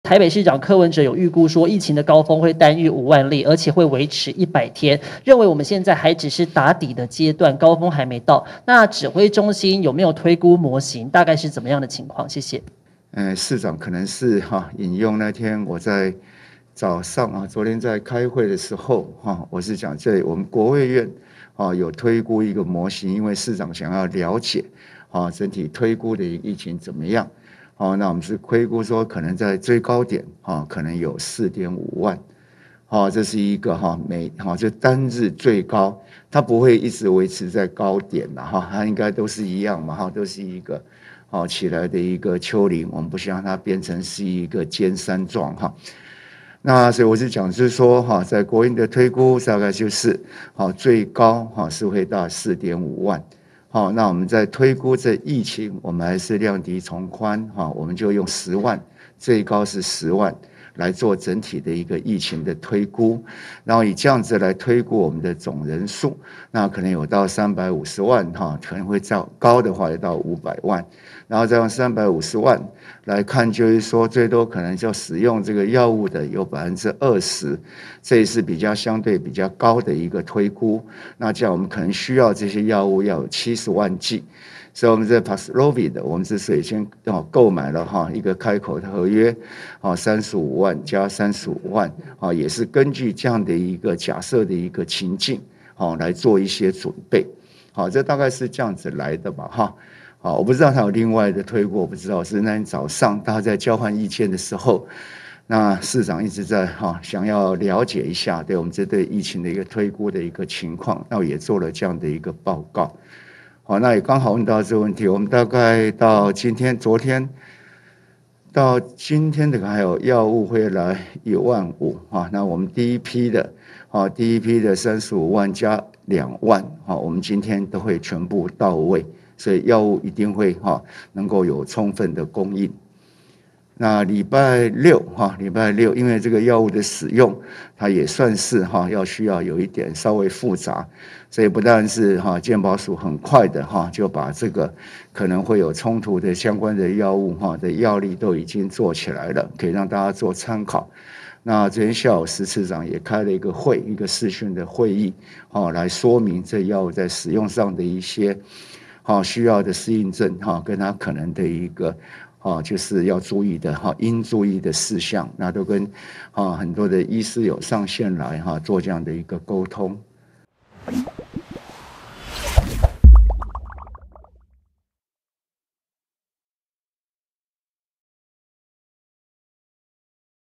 首揭疫情高峰日增4.5萬確診 陳時中：最多500萬人染疫｜中時新聞網
台北市長柯文哲預估的高峰單日5萬日會維持100天，陳時中表示，柯市長是引用前天與國衛院開會時有推估一個模型，推估可能最高點會單日有4.5萬人確診，但不會一直維持在高點，希望模型為丘陵，不希望變成尖山狀，在這樣的推估下，最高會到4.5萬例。

【相關新聞】
陳時中首揭疫情高峰日增4.5萬確診 最多500萬人染疫